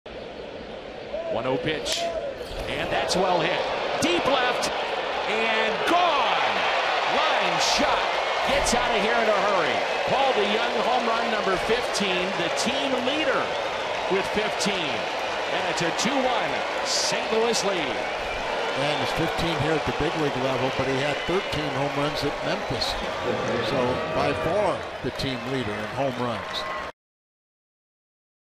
STL@CIN: DeJong belts a two-run smash to left
Paul DeJong hits a two-run home run to left field to give the Cardinals a 2-1 lead in the top of the 3rd inning